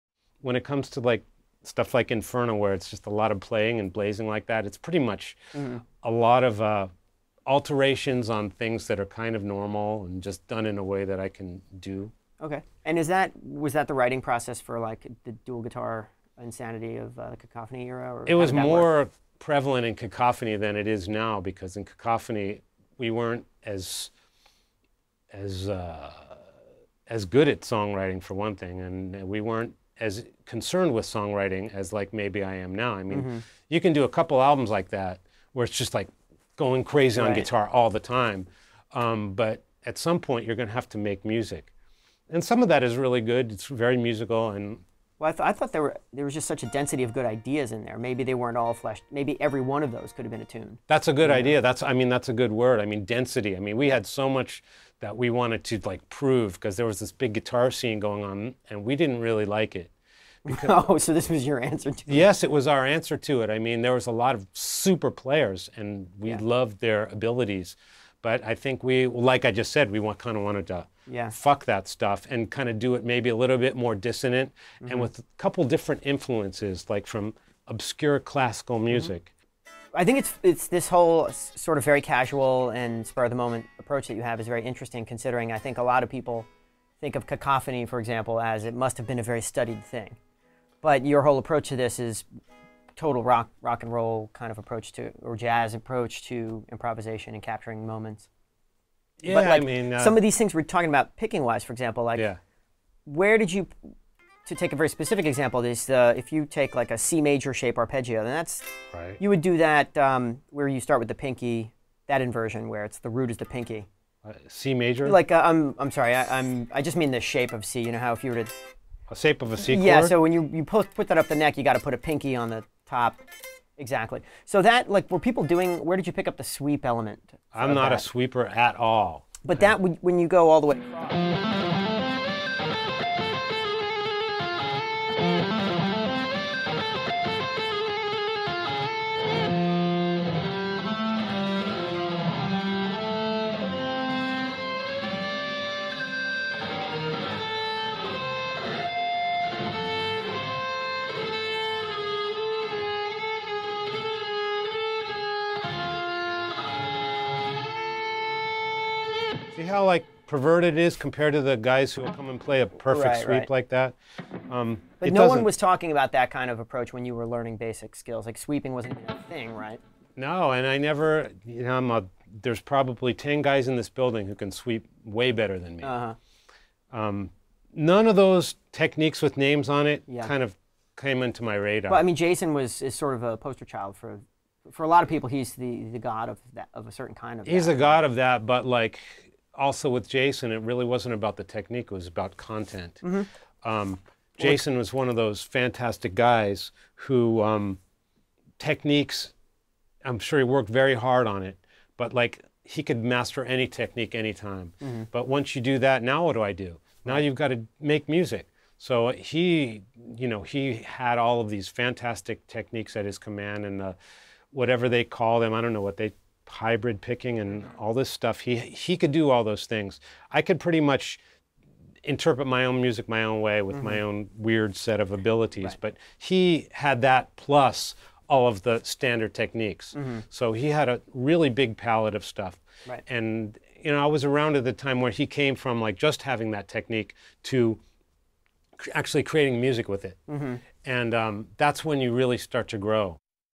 Marty Friedman on Songwriting, Cacophony, and Jason Becker
Marty discusses the dense, dissonant, and diversely-inspired sounds of Cacophony as a response to the prevailing shred scene — and the palette of fantastic techniques that fueled Jason Becker's exploration of musical ideas.